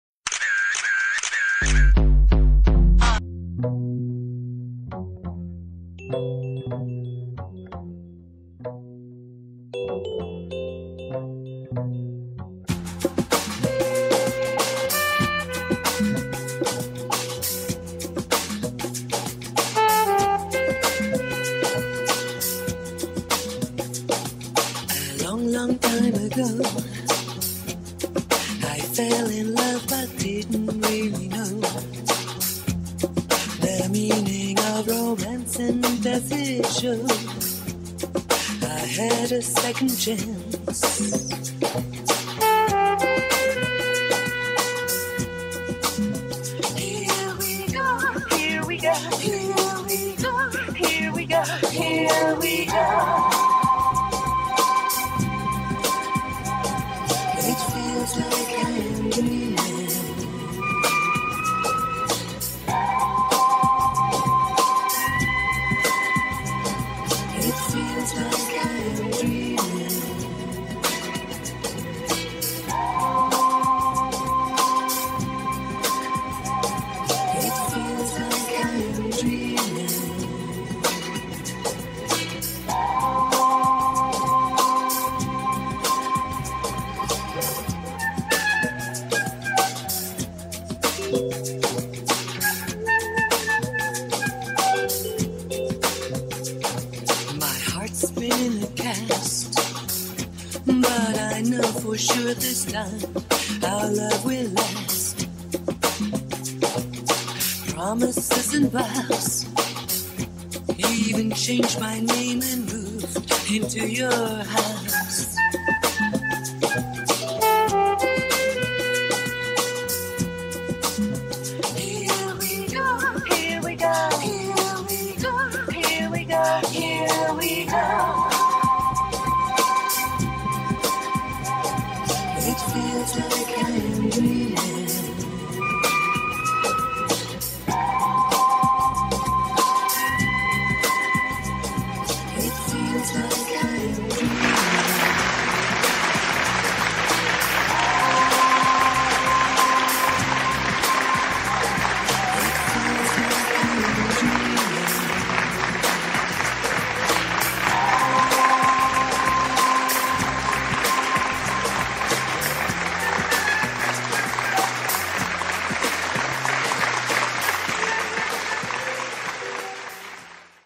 Holiday Looks by Greta Constantine from Toronto Runway
Holiday Looks by Greta Constantine from Toronto Runway, show feminine, elegant designs.